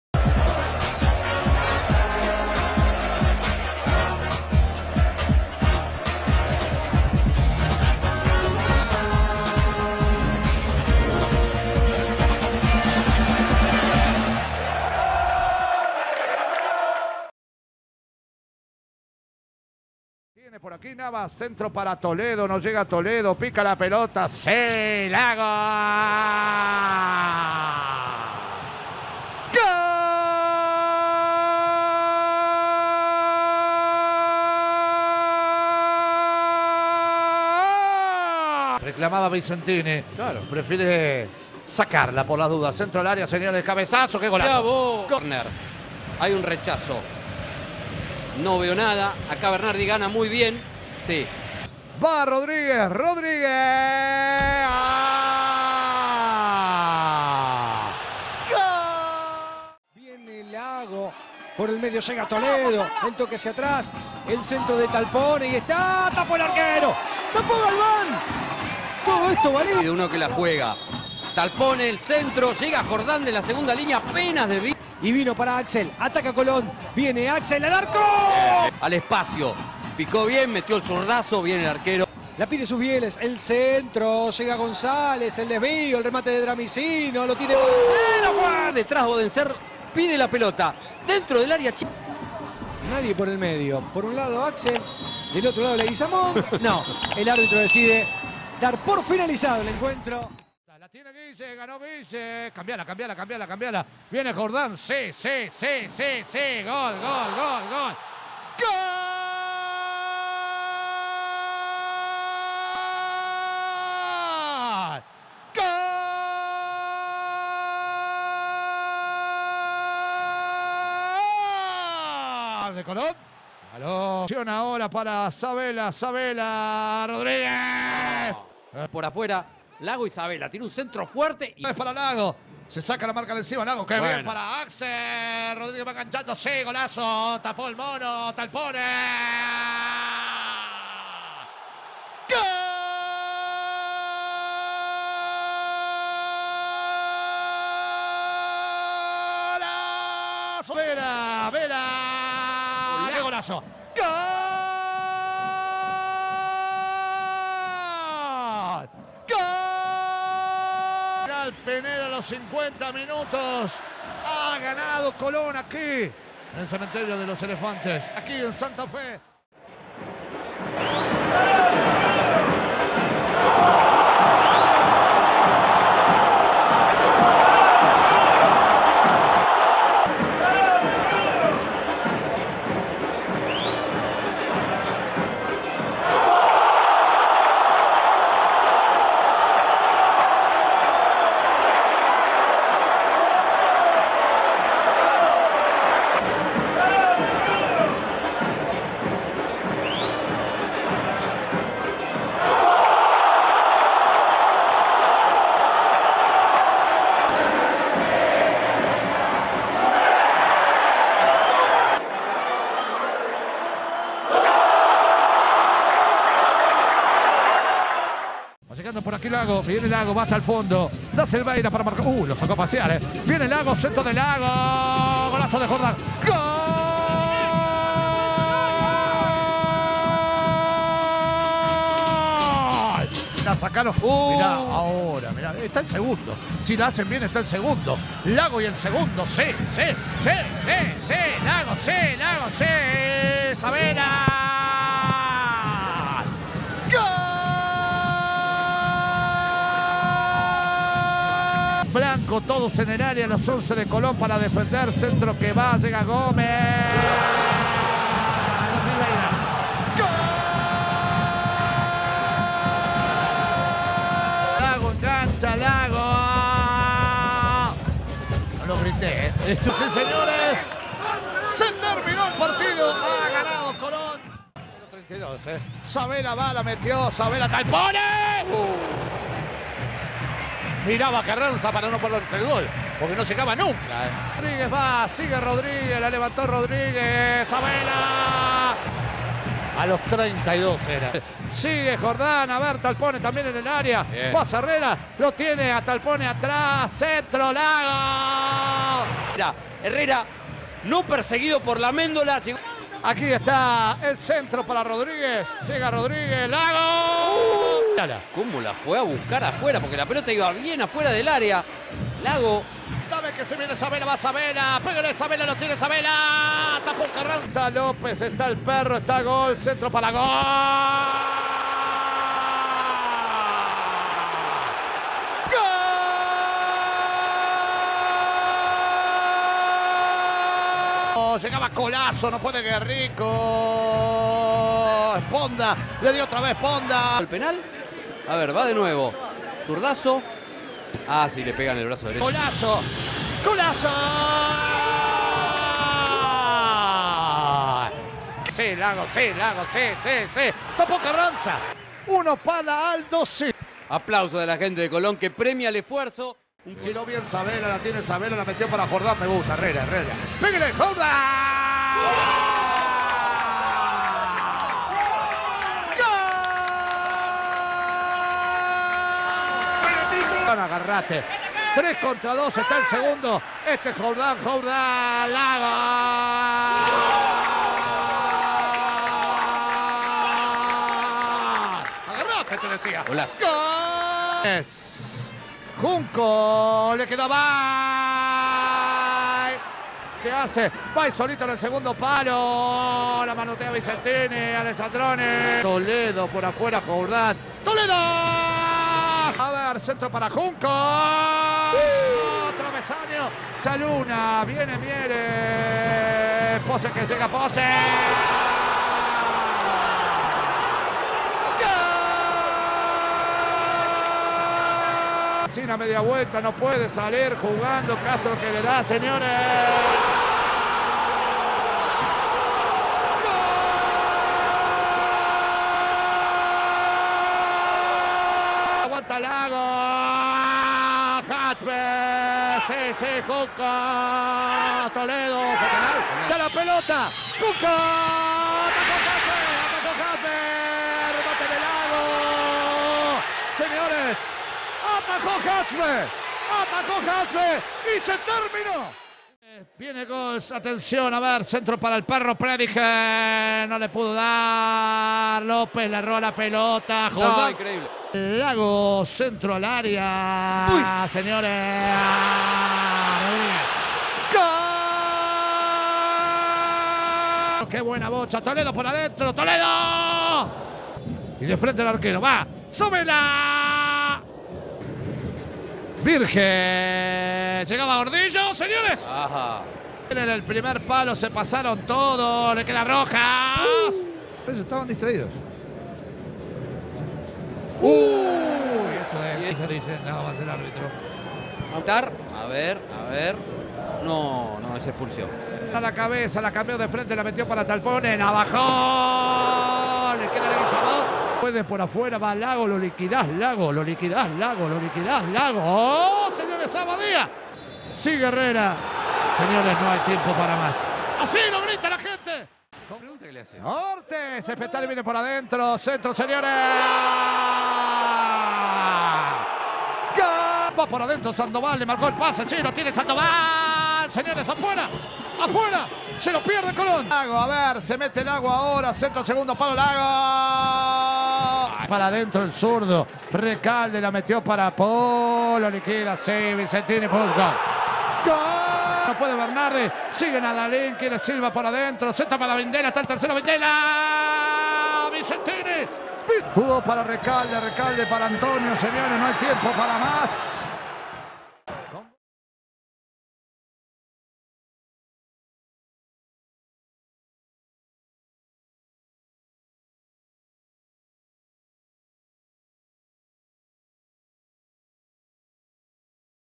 EL COMIENZO INVICTO DE COLÓN EN LA B NACIONAL 2024
Este es un resumen de las primeras 9 fechas que disputó Colón en la B Nacional 2024, de las cuales, las primeras 8 no perdió en ninguna. El finde pasado, Colón perdió el invicto que mantenía junto a Quilmes.

Gracias a los que apoyan el canal con su membresía:
Emanuel Valdes